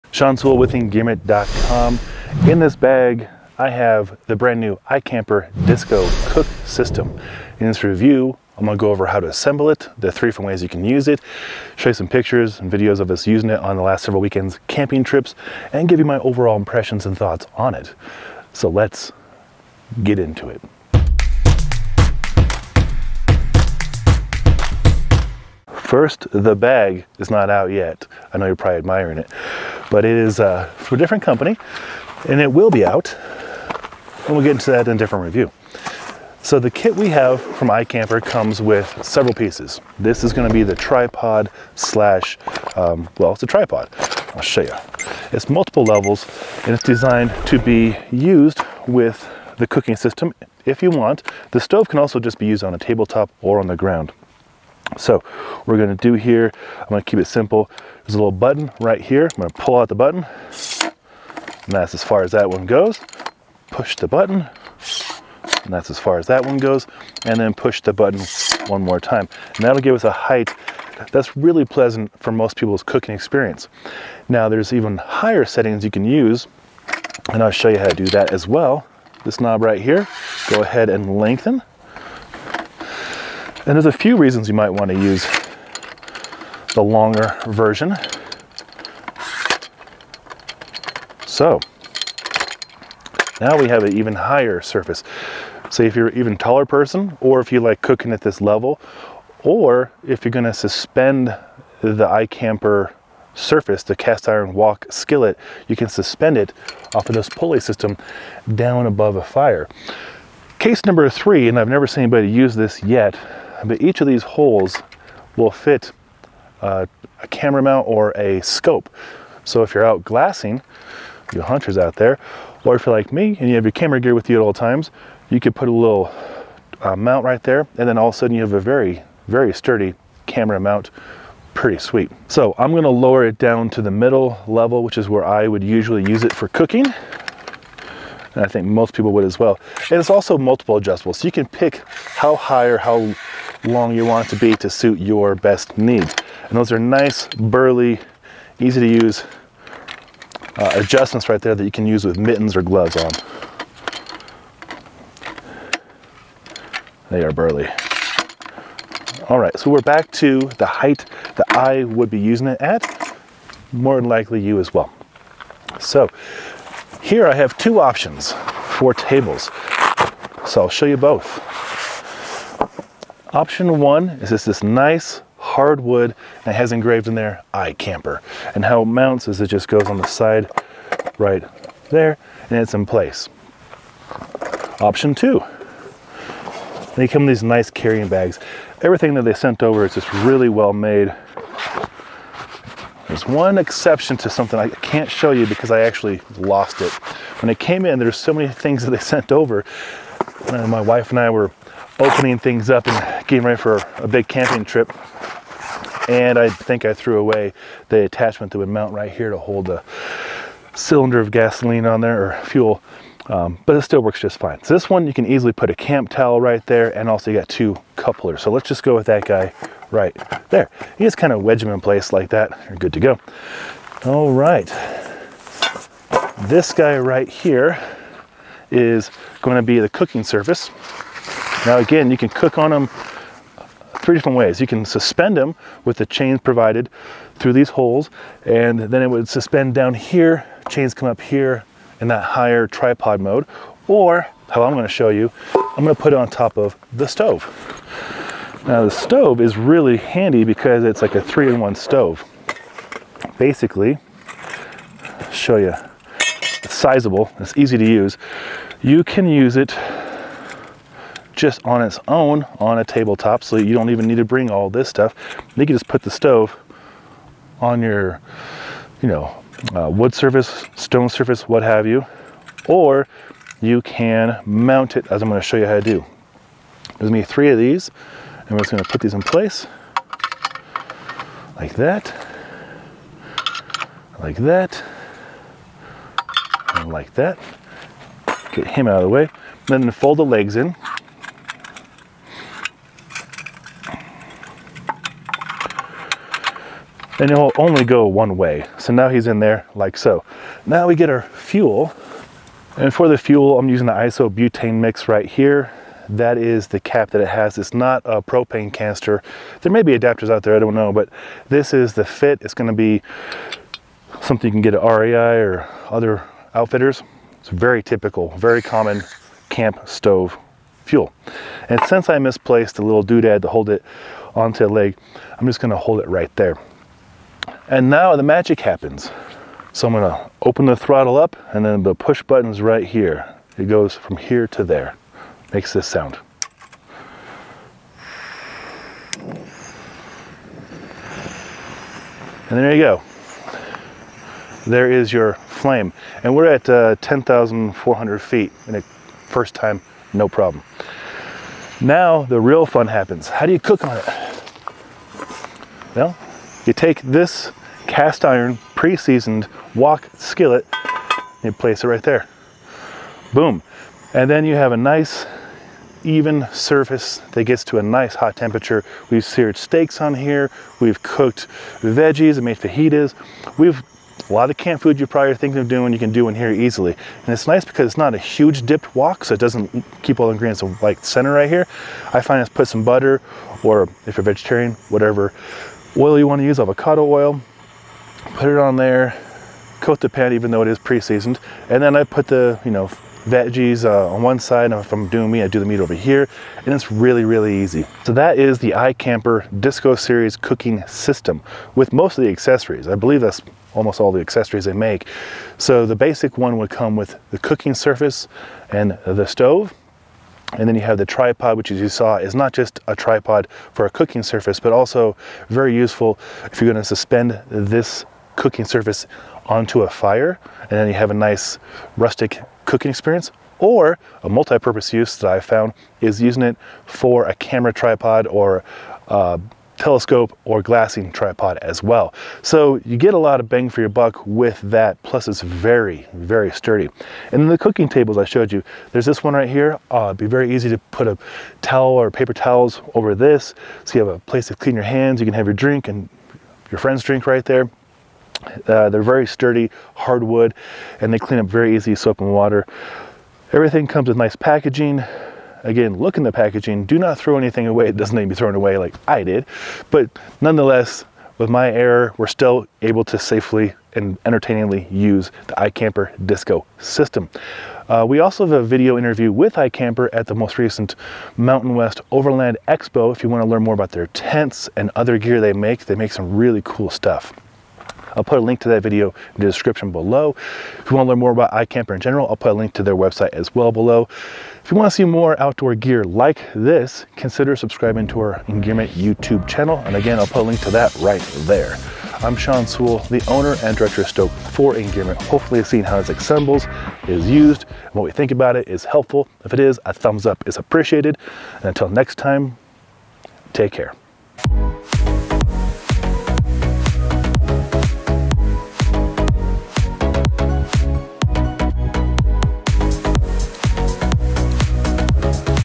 iKamper Disco Series Stove and Starter Kit - 3 Ways to Cook!
The iKamper Disco Series Stove and Starter Kit is a really nice set up for gourmet camping chefs.  The cast iron skillet / wok is preseason and ready for cooking.  We did up steaks, veggies, burgers, brats, pizza and more on the set up.

I like that the stove can be used on table top, or on various heights.  Even over a fire using the pully system.